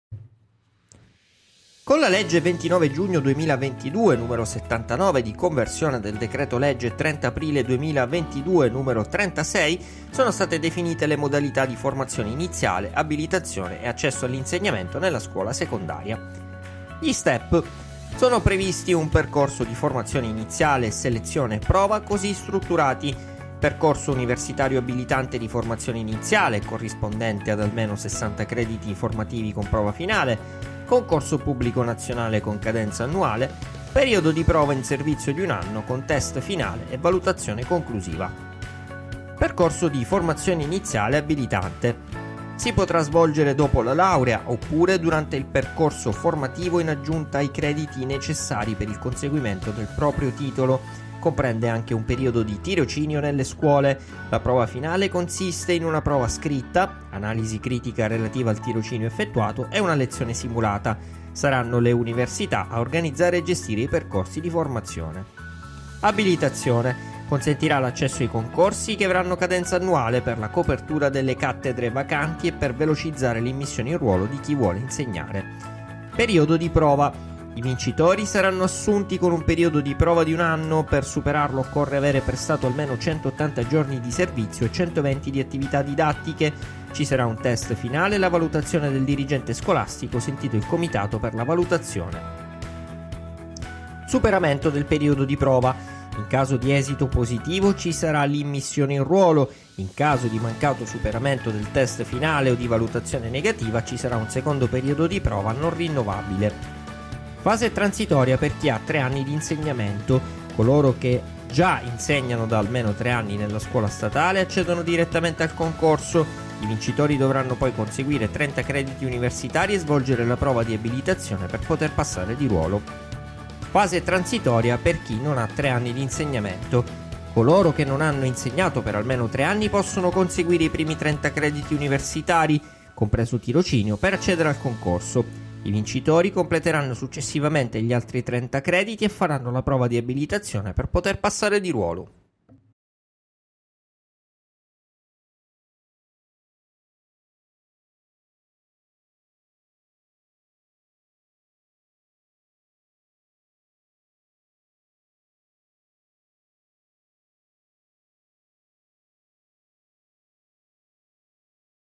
Come diventare insegnante? La guida completa con le nuove indicazioni del Ministero
docenti tecnicadellascuola Come si diventa insegnanti secondo la nuova legge 79/2022 di conversione del DL 36 sul reclutamento e la formazione docenti? Quali le principali novità della legge in fatto di immissioni in ruolo?
Ecco una guida che aiuterà i futuri docenti nell'iter di reclutamento.

e sui nostri canali social